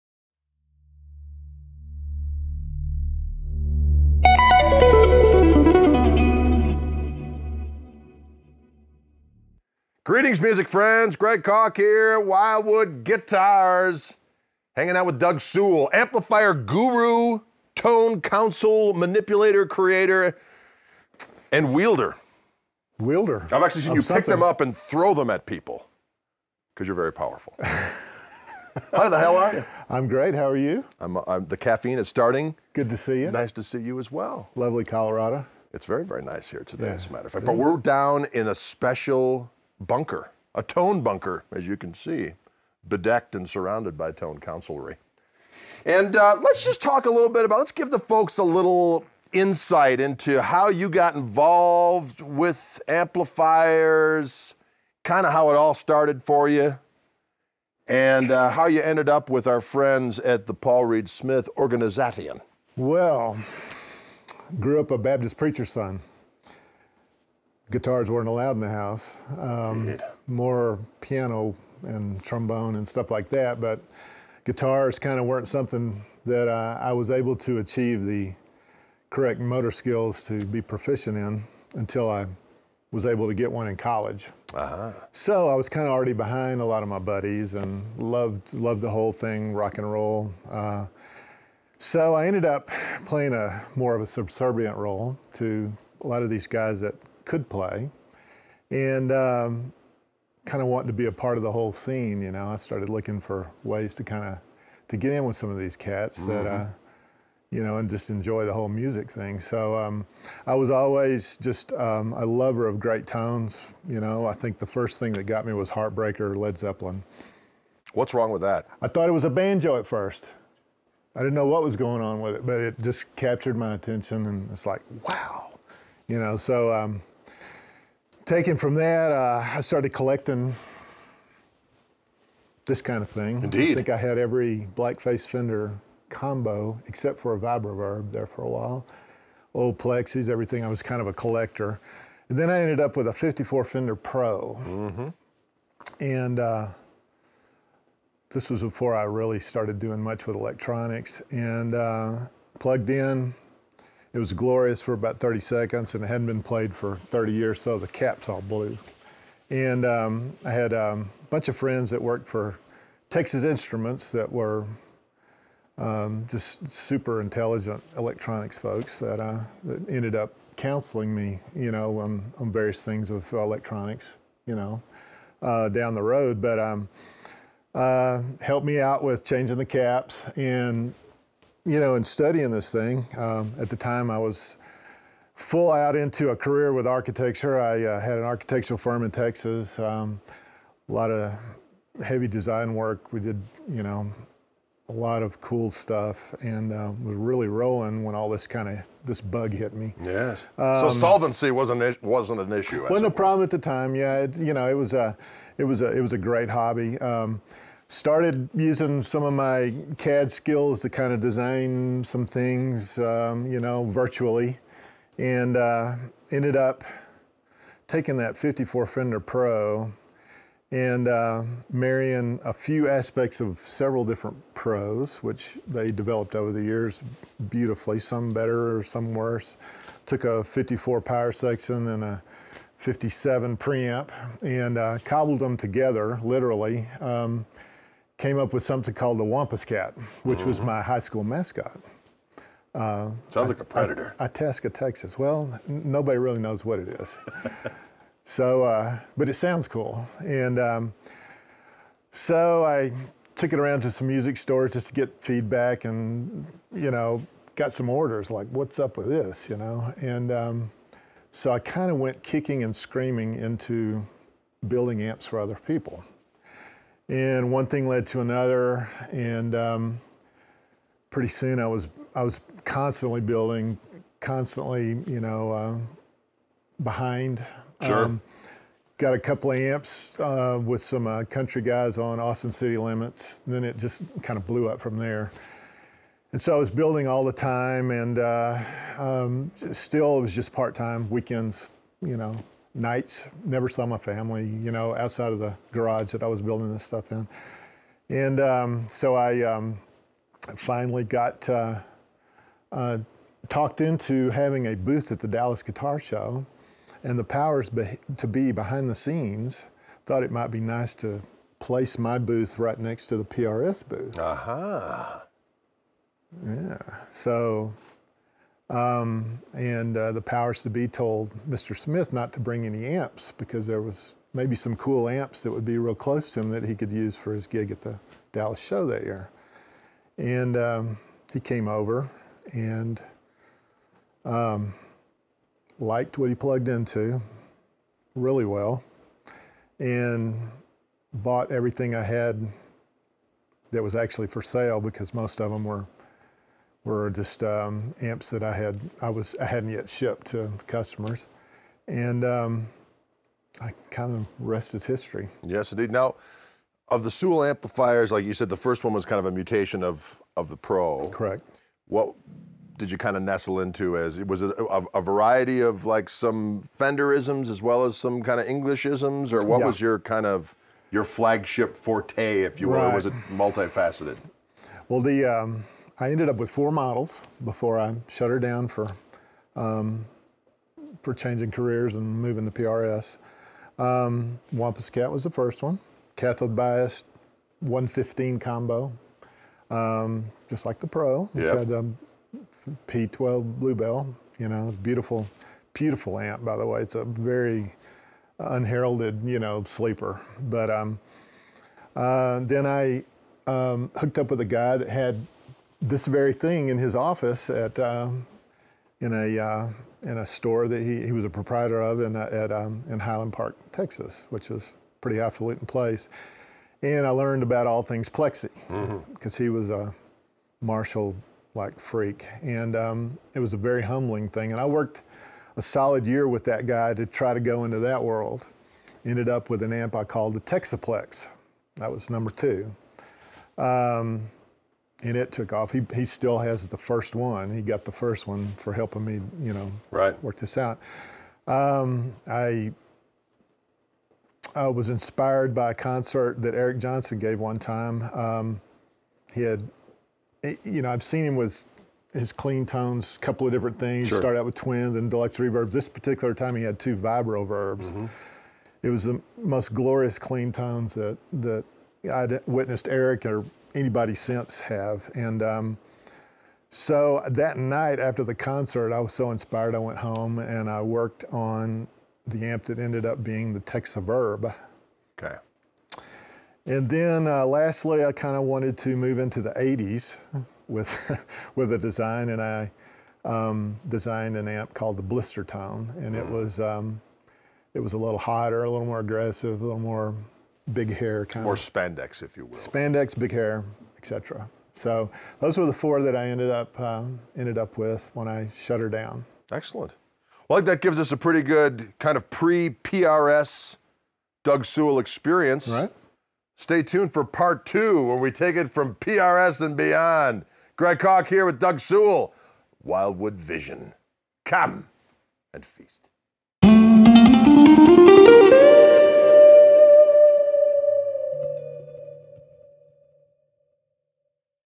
PRS' Doug Sewell & Greg Koch On Doug's History • Wildwood Guitars
Great deeds always begin with humble beginnings.  Join Greg Koch and Wildwood Guitars as we sit down with PRS Amp mastermind Doug Sewell to discuss his early years as an amp designer!

~ Doug Sewell ~
The pursuit of passion can drive many a man to turn his back on all that he's come to know in hopes of coming one step closer to the sweet release of creative satisfaction.  Doug Sewell of PRS amps knows that story all too well.  Once a prominent architect, the idea of making a career out of designing amplifiers was always just a fantasy.  But as time went on, and the tone continued to leak from his engineered fingertips, Doug could only hold on tight as his own small-time amp company turned into a full-fledged amp revolution.

~ Greg Koch ~
Hailing from the frigid wasteland of northern Wisconsin comes a beast the likes of which the world has never known.  Greg Koch, a beast of both persona and guitar-mastery, couples his sickeningly magnetic personality with the guitar to create a style of playing that is both completely unique and deplorable at the same time.  As a huge fan of the legendary Big Jim Hendricks Greg has taken all that is good and righteous about blues guitar and warped it into a ridiculously fantastic amalgamation of melodic defilement and mind-blowing creativity.  Over the years Greg has shared the stage with everyone from Joe Bonamassa to Les Paul himself, never relenting, never ceasing to bring the ruckus.